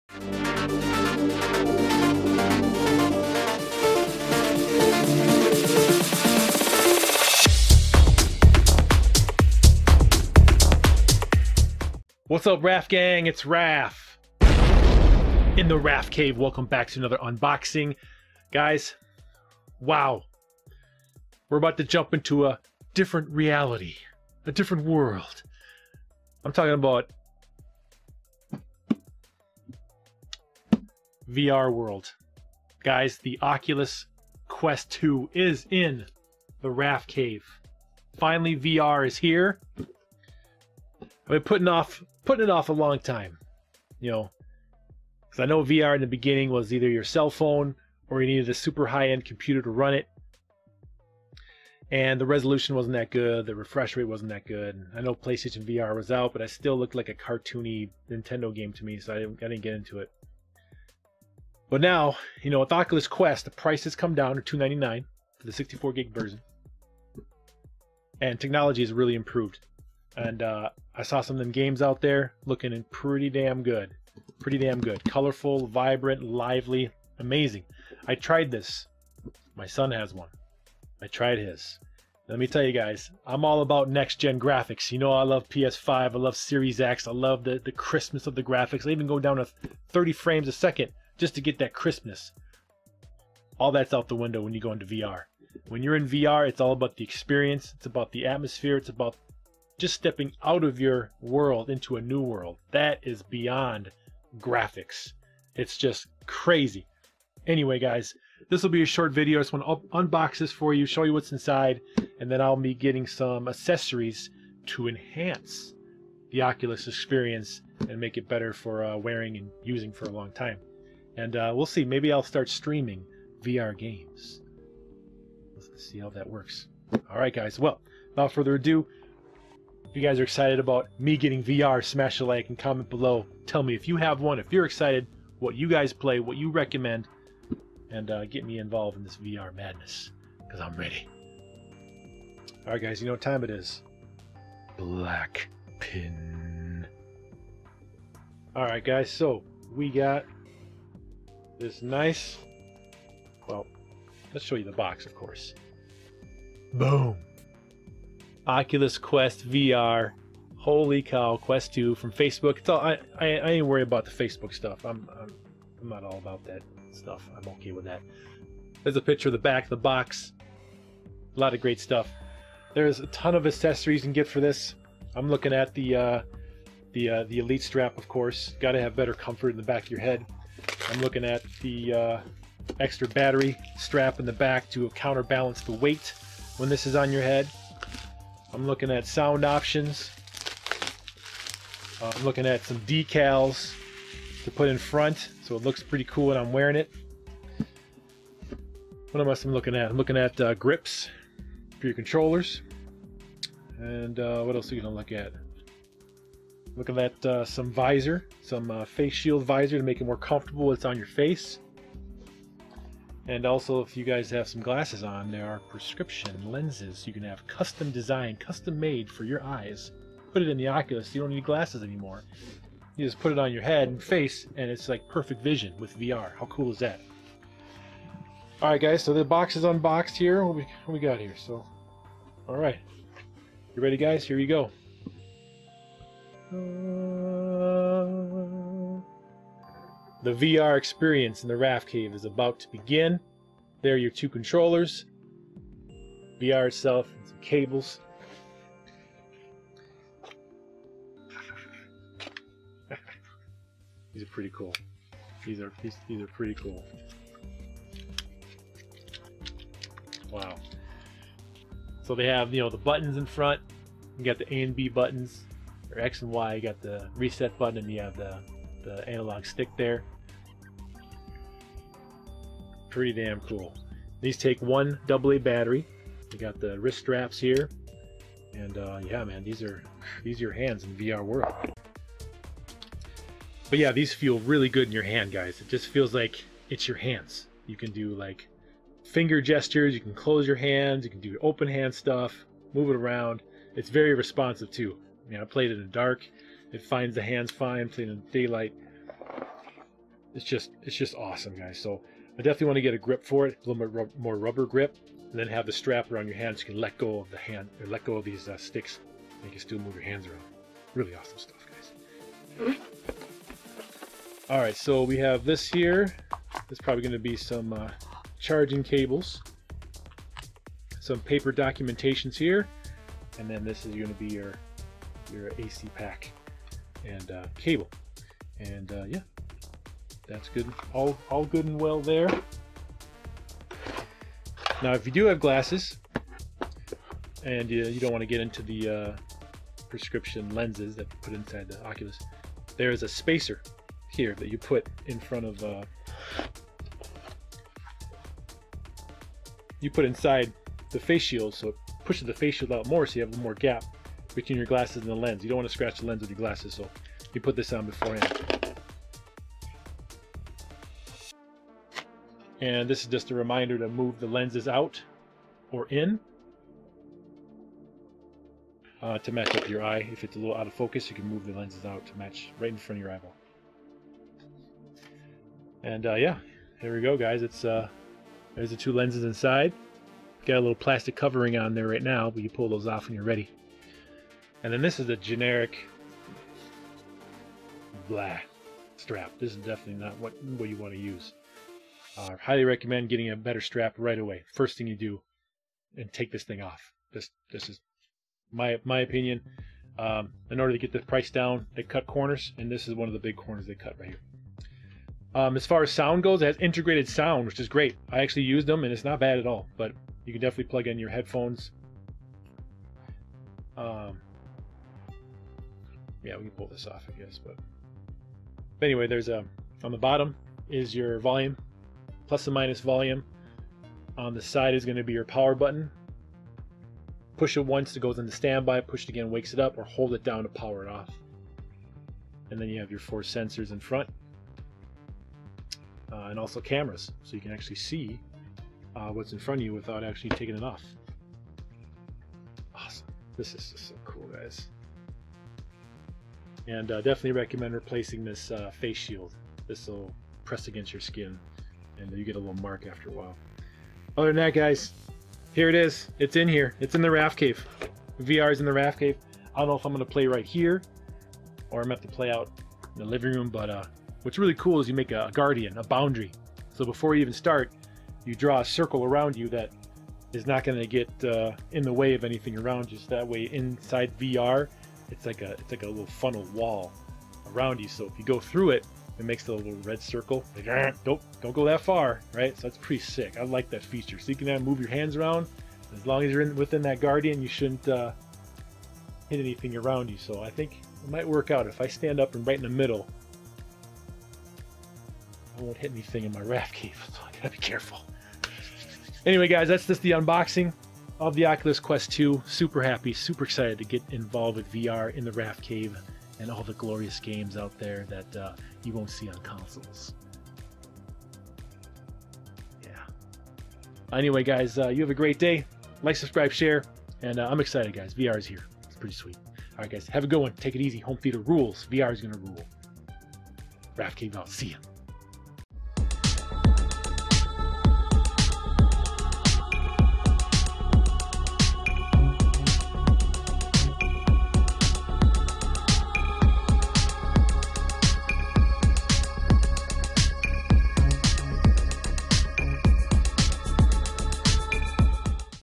I got an Oculus Quest 2! : UNBOXING
The Rafcave Is now VR Compatible!

Grab one here from Amazon -

Or get now pay later with Abunda -

Get your Rafcave Gear from the TeeSpring Store -